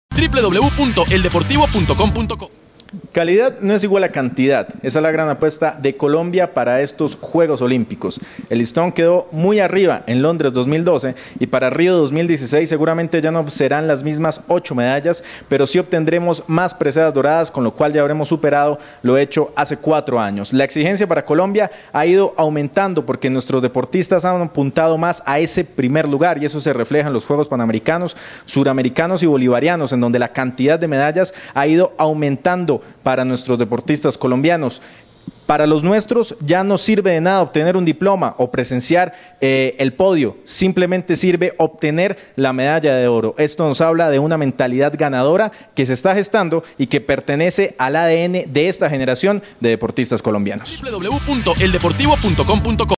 Colombia dio el salto de calidad
Lo que está sucediendo en los Juegos Olímpicos de Río 2016 ha superado las expectativas y las sorpresas podrían ser mayúsculas.

Videocolumna: Julián Angarita
Producción: William Cortés